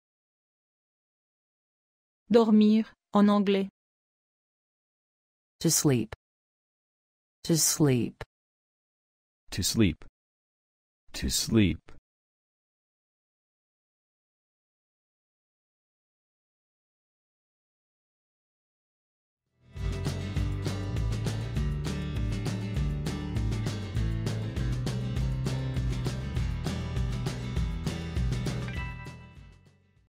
Comment dire dormir en anglais
Comment dire correctement dormir en anglais? Ecoute avec nous.
Quelle est la traduction correcte du mot dormir du français vers l'anglais? Parlez comme les Américains. Aussi, comment écrire correctement le mot to sleep?